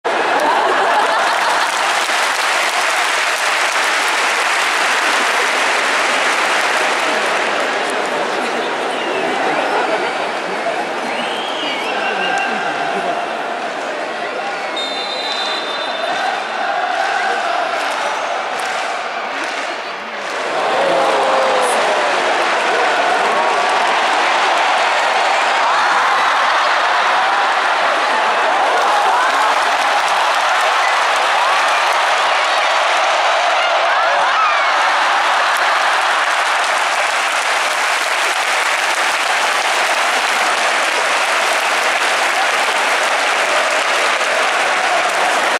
浦和レッズ 山田暢久引退試合 前半FK①からワシントンシュート
2014.7.5　山田暢久引退試合　浦和レッズvsレッズ歴代選抜　埼玉スタジアム2002